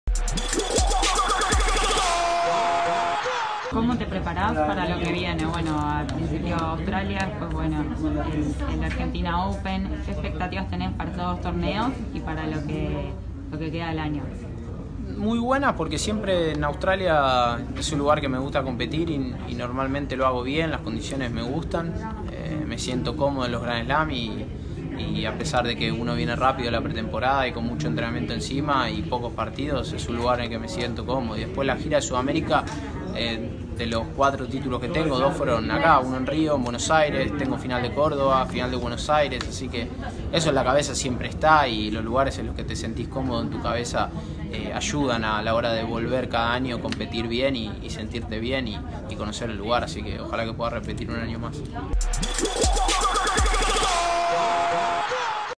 Cómo se prepara Diego Schwartzman para el Grand Slam de Australia
El Peque se refirió al Australian Open en charla con Olé.